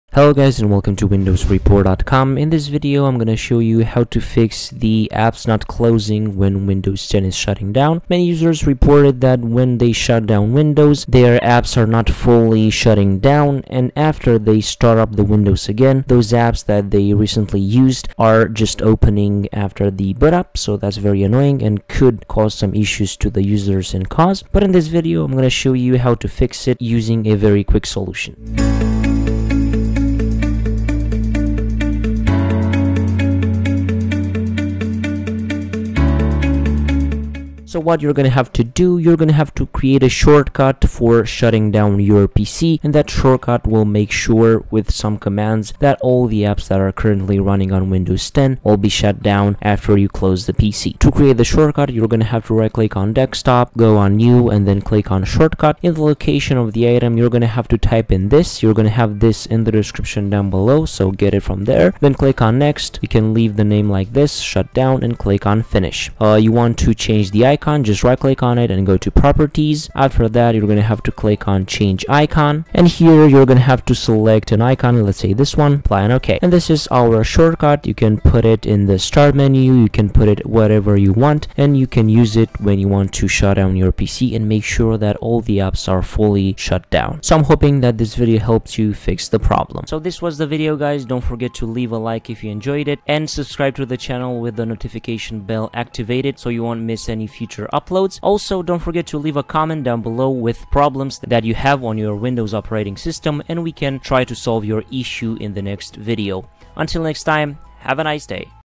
2024 Guide to Fix: Windows 10 not closing Apps when shutting down
| ⚙️Try Fortect to fix and optimize your PC like a Pro

In this video, we're going to show you how you can quickly fix Windows 10 not closing apps when shutting down using a very quick solution. The problem has been reported by a lot of users, who reported that when shutting down windows 10, the operating system won’t fully close the apps that were open, and at the next start up, Windows will load app the same apps that were open at the last shut down. The problem can be easily fixed by creating a shortcut that you can use to shut down the PC, and like that Windows will surely fully close all the apps that were open at the time.

How can I fix Windows 10 not closing apps when shutting down ?
Solution : Create a Shutdown Shortcut : 0:37

Command used in the video :
%WINDIR%\system32\shutdown.exe /s /hybrid /t 0

In this video, we cover the following issues:
1. Windows 10 not closing apps when shutting down
2. Apps not fully closing when Windows 10 is shutting down
3. Windows 10 loading apps that were open at the last shut down
4. Windows 10 opening browser with same tabs after boot up
5. Recent used apps launch at start up